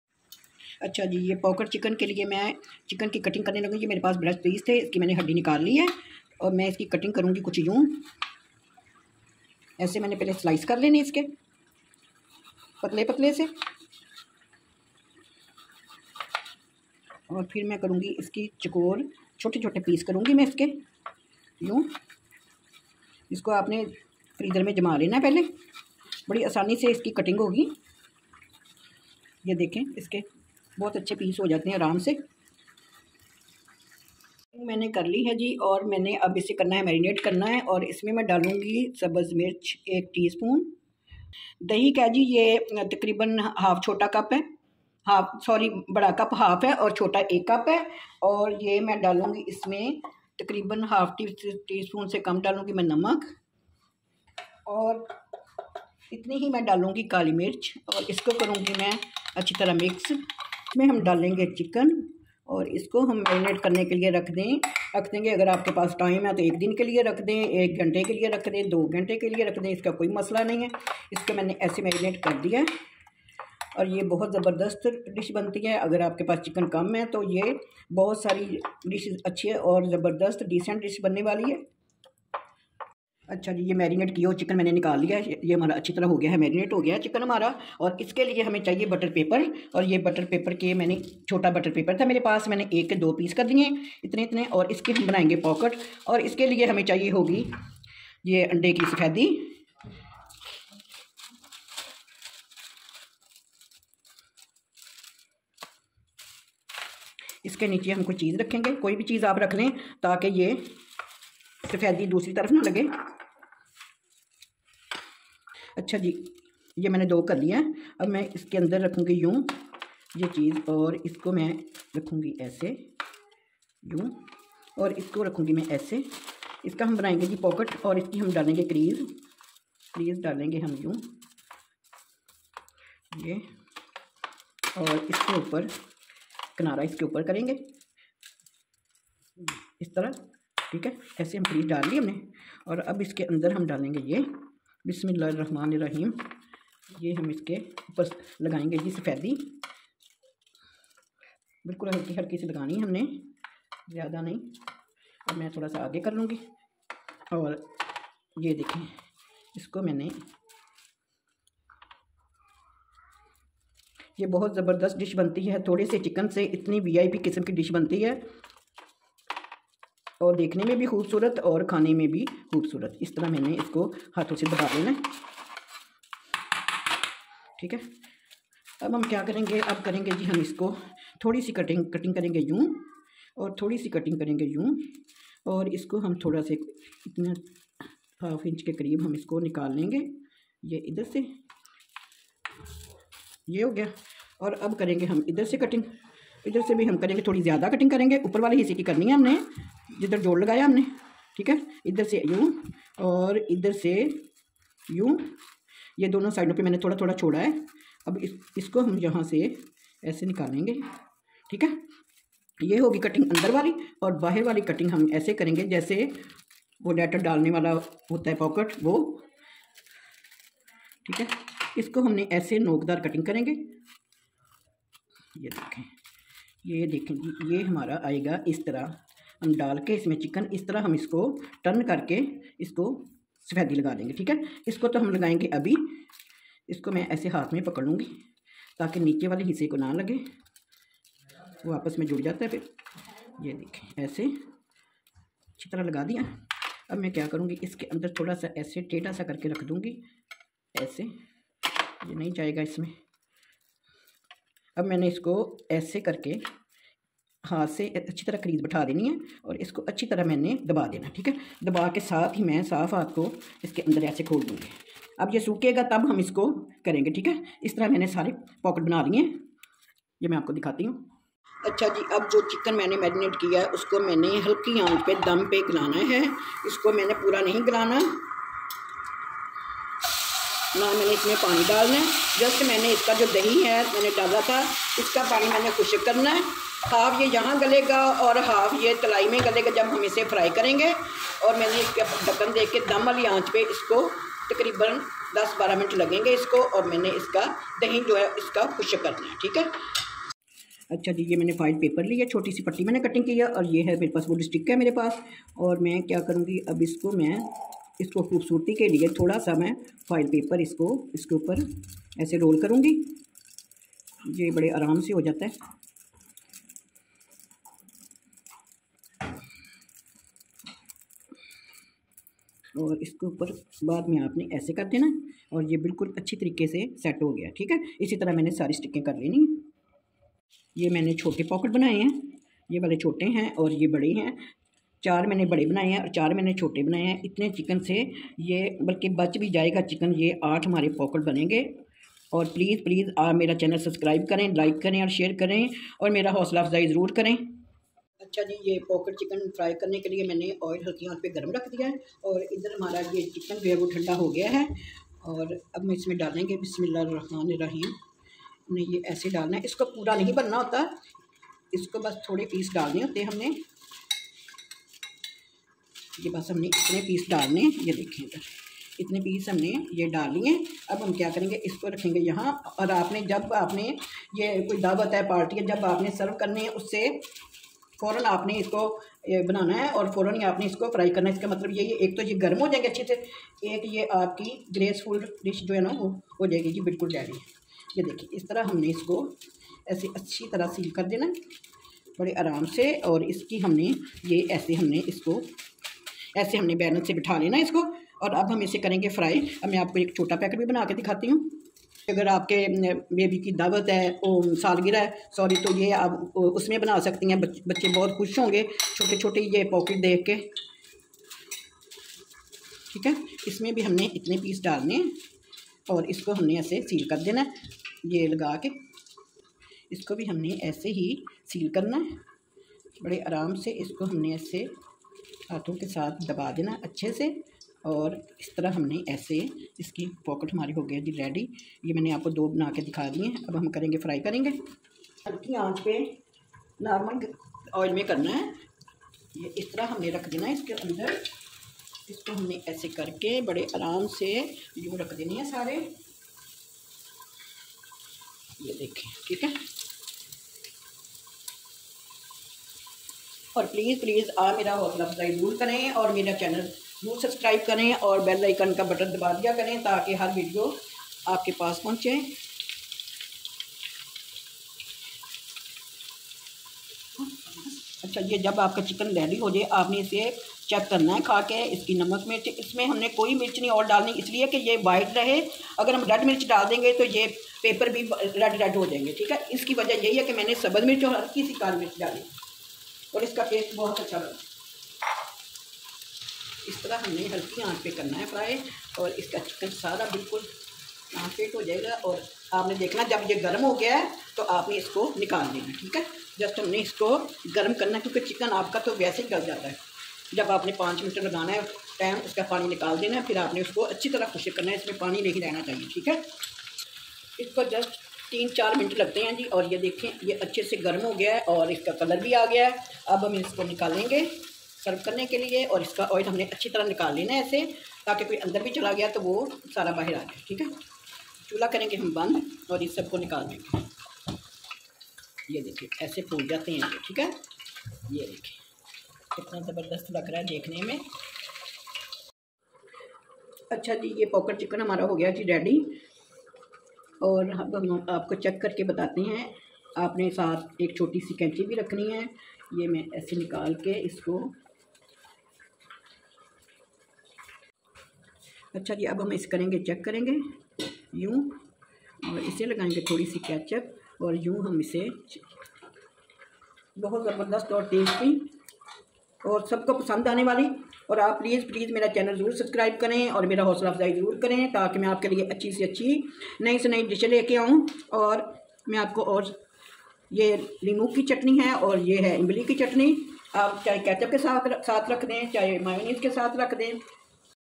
How to make delicious pocket chicken recipe? | Easy And Different recipe by BK COOKING
Ingredients:
Chicken
Yogurt 1 small cup
Green chilli 1 tsp